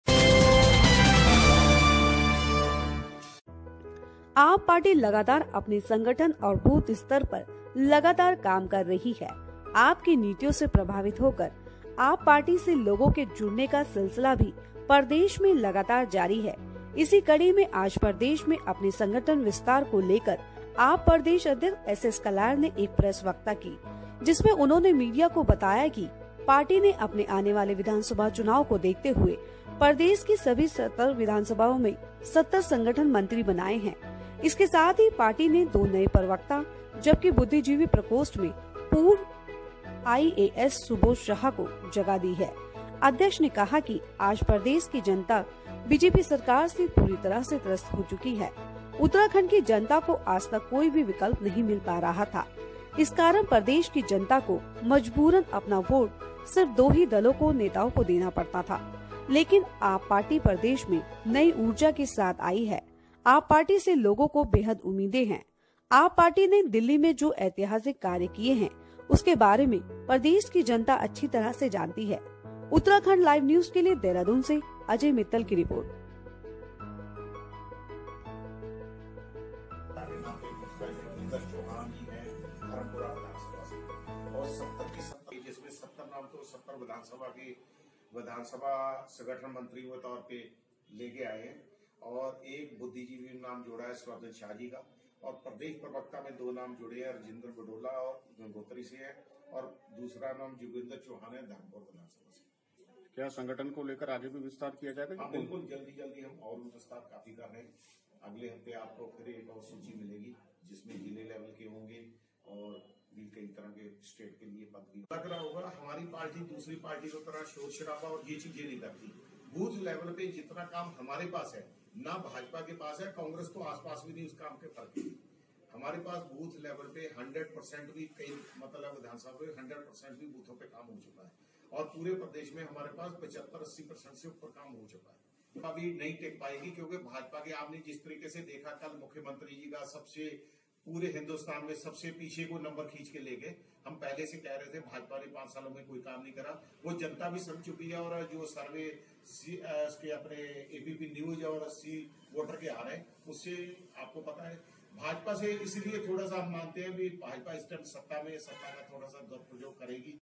आप पार्टी PC
आप पार्टी लगातार अपने संगठन और बूथ स्तर पर लगातार काम कर रही है आप की नीतियों से प्रभावित होकर आप पार्टी से लोगों के जुड़ने का सिलसिला भी प्रदेश में लगातार जारी है इसी कड़ी में आज प्रदेश में अपने संगठन विस्तार को लेकर आप प्रदेश अध्यक्ष एस एस कलेर ने एक प्रेस वार्ता की जिसमें उन्होंने मीडिया को बताया कि पार्टी ने आने वाले विधानसभा चुनावों को देखते हुए प्रदेश की सभी 70 विधानसभाओं में 70 संगठन मंत्री बनाए हैं। इसके साथ ही पार्टी ने 2 नए प्रवक्ता जबकि बुद्विजीवी प्रकोष्ठ में पूर्व आईएएस (IAS) सुर्बधन शाह को जगह दी है। अध्यक्ष ने कहा कि आज प्रदेश की जनता BJP सरकार से पूरी तरह से त्रस्त हो चुकी है। उत्तराखंड की जनता को आज तक कोई भी विकल्प नहीं मिल पा रहा था। इस कारण प्रदेश की जनता को मजबूरन अपना वोट सिर्फ दो ही दलों के नेताओं को देना पडता था। लेकिन आप पार्टी प्रदेश में नई ऊर्जा के साथ आई है। आप पार्टी से लोगों को बेहद उम्मीदें हैे। आप पार्टी ने दिल्ली में जो एतिहासिक कार्य किए हैं उनके बारे में प्रदेश की जनता अच्छी तरह जानती है।उत्तराखंड लाइव न्यूज़ के लिए देहरादून से अजय मित्तल की रिपोर्ट।